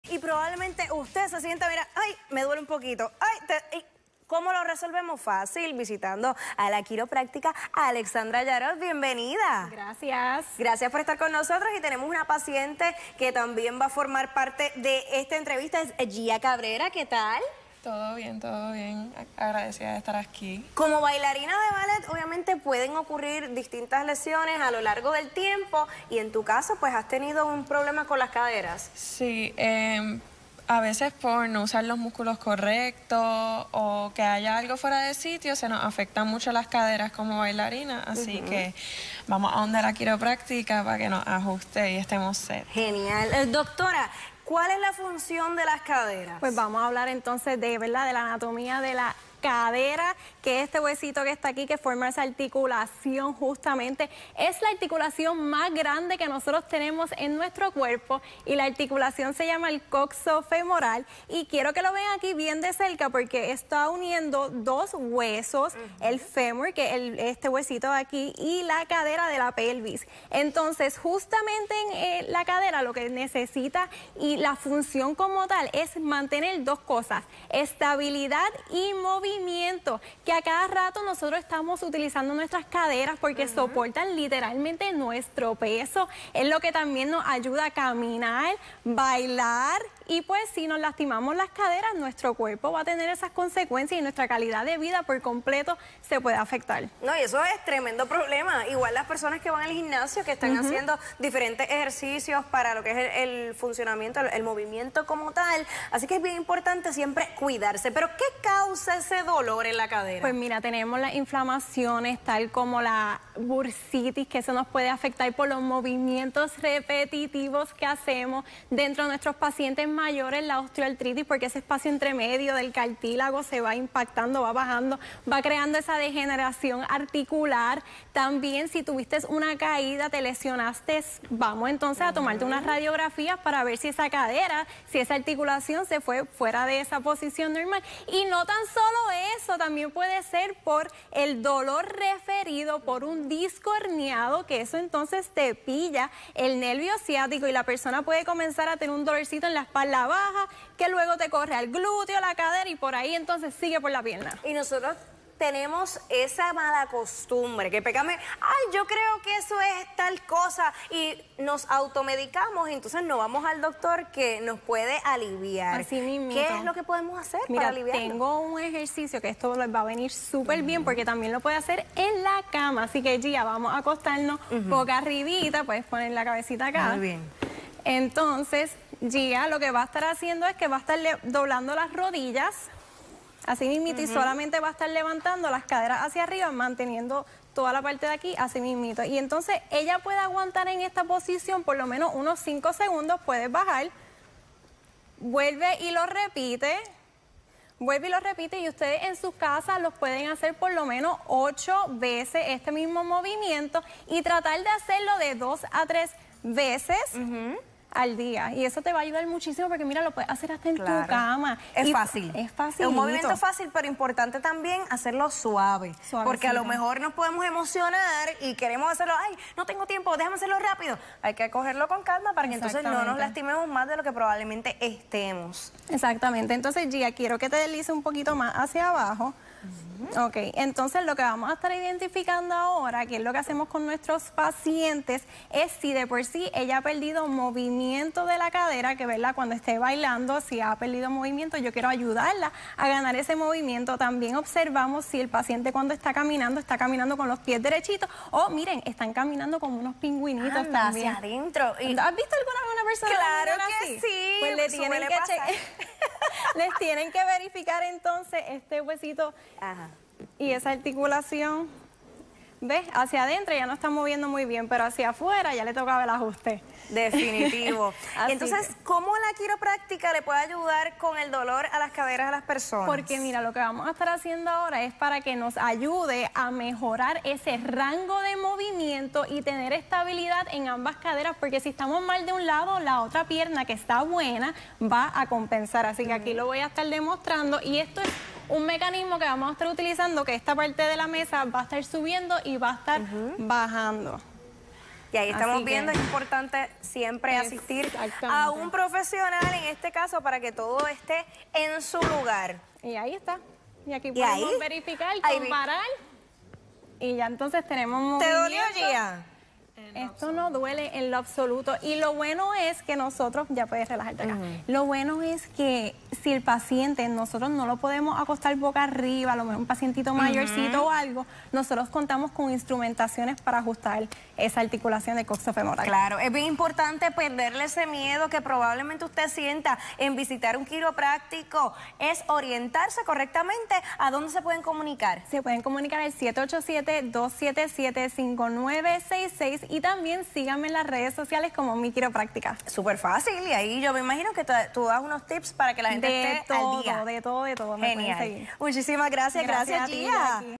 Truquitos para aliviar los dolores de cadera
Quiropráctica explica a qué se deben.